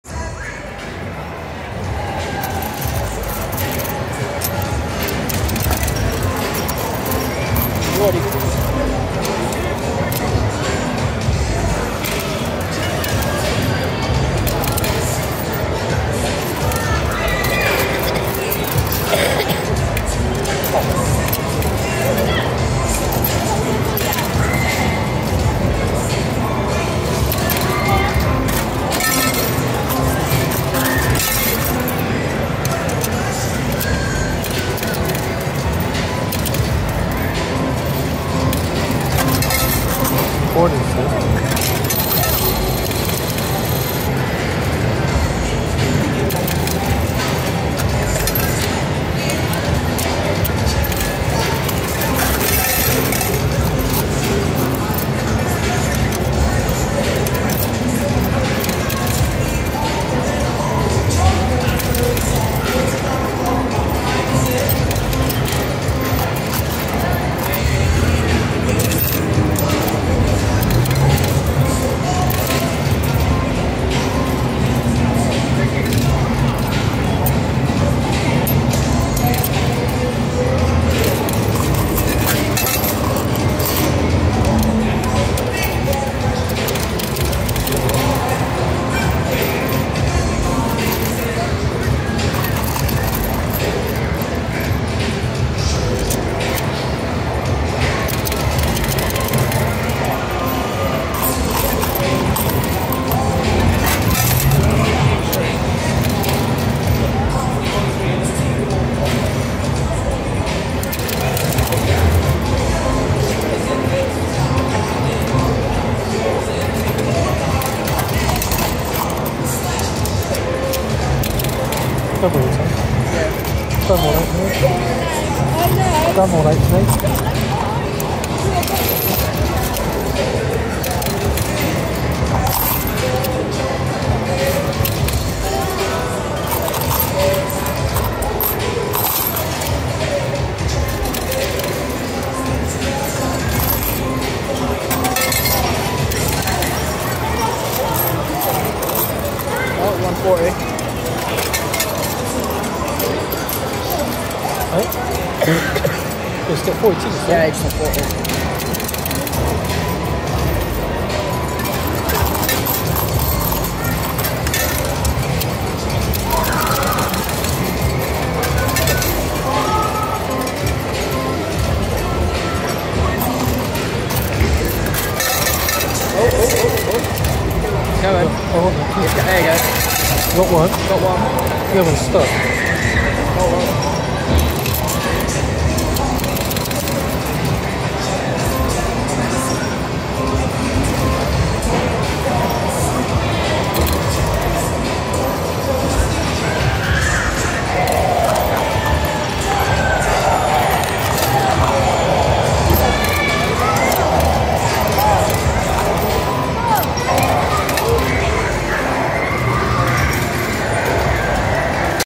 Playing On The Strike Ten 2p Coin Pusher On The Grand Peri
Playing on the Strike Ten 2p coin pusher in the arcades on the Grand Pier, Weston Super Mare